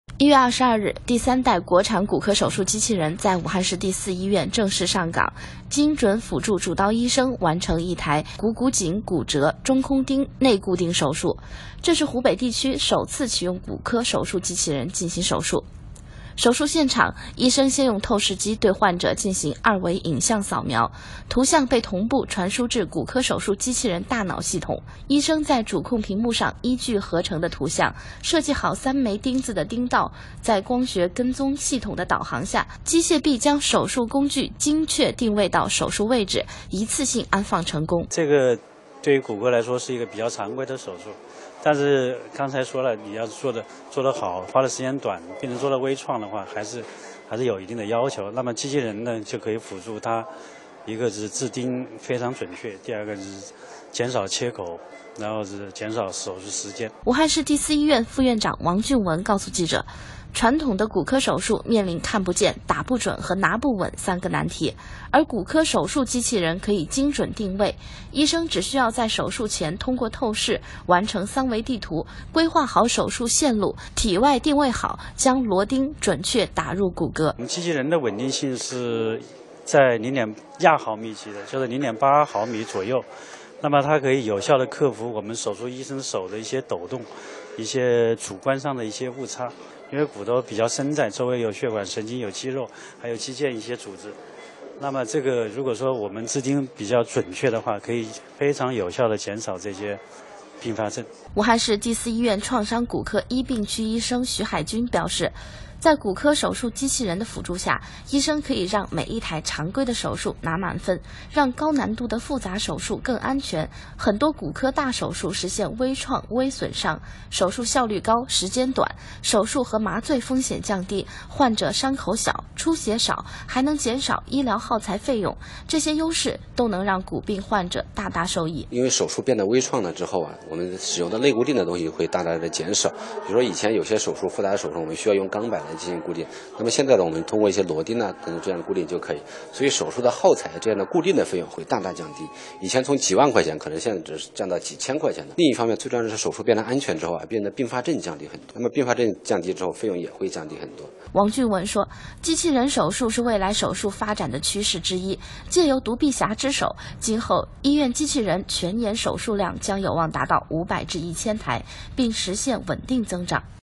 机器人“操刀” 骨科手术 误差小于0.8毫米
1月22日，第三代国产骨科手术机器人在中国湖北省武汉市第四医院正式“上岗”，精准辅助主刀医生完成一台股骨颈骨折中空钉内固定手术，这是湖北地区首次启用骨科手术机器人进行手术。传统的骨科手术，面临看不见、打不准和拿不稳三个难题，而骨科手术机器人可以精准定位，医生只需要在手术前通过透视，完成“三维地图”，规划好手术线路，体外定位好，将螺钉准确打入骨骼。